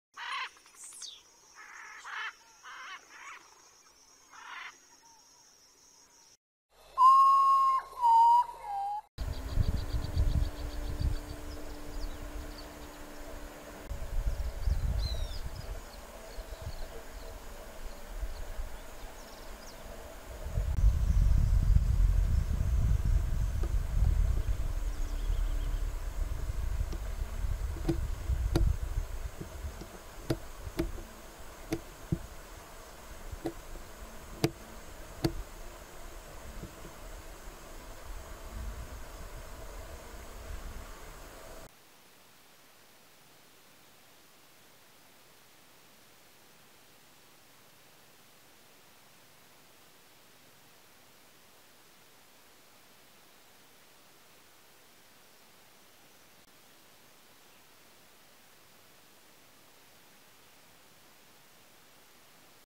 Golondrina ala rasposa sureña (Stelgidopteryx ruficollis) Southern Rough-winged Swallow
Características
El adulto es de 13,5 cm en largo y pesa 15 g; marrón arriba, con alas y cola negruzcas y el obispillo de un gris pálido. La garganta y la parte superior del pecho son de color herrumbre con las partes inferiores bajas de un blanco amarillento. La cola se le bifurca levemente.

Es similar en aspecto a sus contrapartes septentrionales, pero la golondrina gorgirrufa es sedentaria y su color es más uniforme, especialmente en la rabadilla.

Hábitat
Se encuentra en zonas abiertas y en los claros del bosque. Anida en las cavidades forradas de diversos tipos de hierba, incluyendo los agujeros en los bancos o las paredes, o los nidos en desuso del martín pescador y el jacamar. No forma a colonias. La nidada es de 3-6 huevos blancos, incubados por la hembra de 16 a 18 días y otros 13 días de cría.

La golondrina gorgirrufa se alimenta de insectos en vuelo, generalmente volando bajo con un vuelo deliberadamente lento. La llamada es un chirrido inarmónico. El borde sus plumas primarias externas es serrado; esta característica es solamente evidente al sostener esta ave.